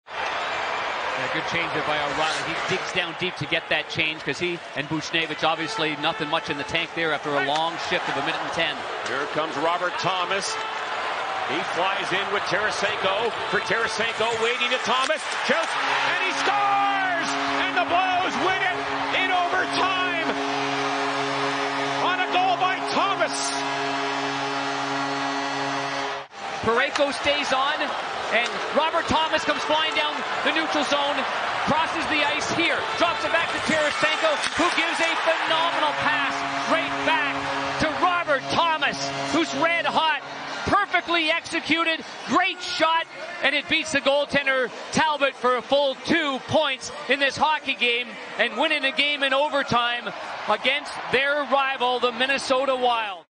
Vladimir Tarasenko sets career high in assist with a dish to Thomas's OT goal vs Wild (8 apr 2022)
Владимир Тарасенко установил рекорд по голевым пасам в одном сезоне - 37.